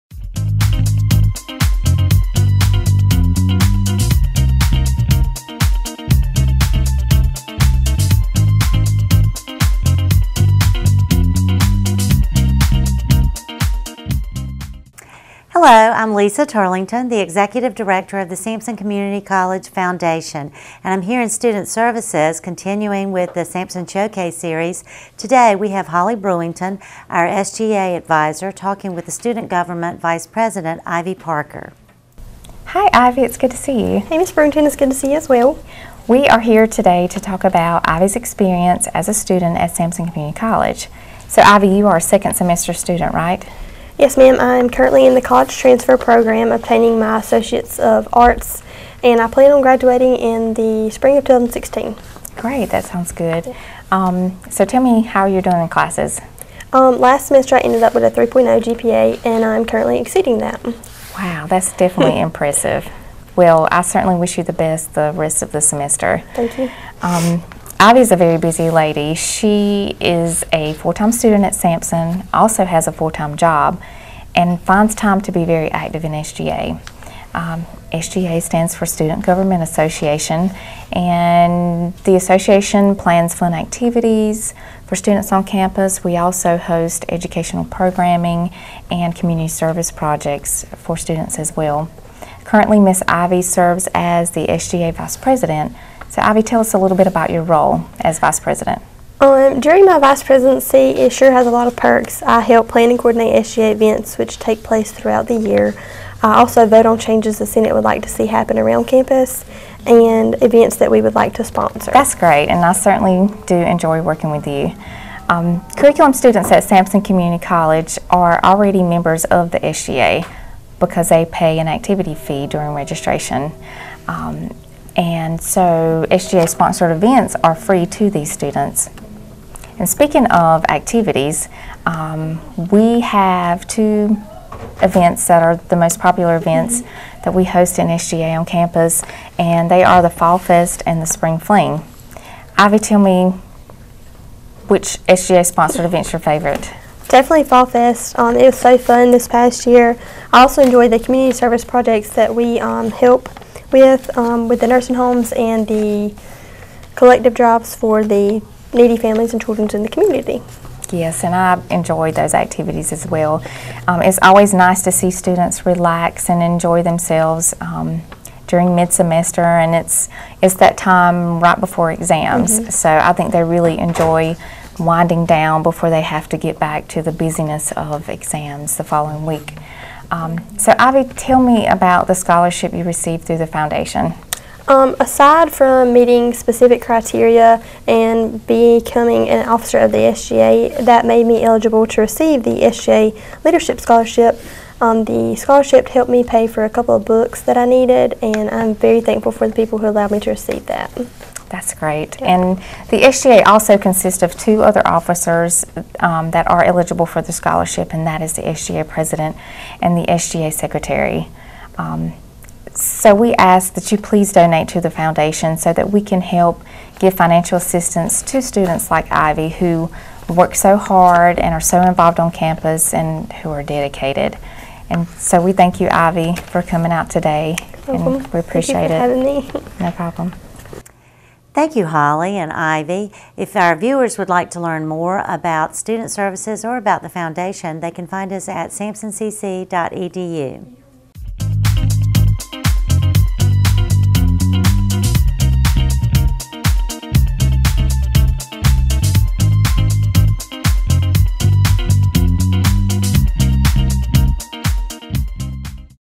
Sampson Showcase presented by SCC Foundation
Lisa Turlington hosts. Holly Brewington interviews Ivey Parker.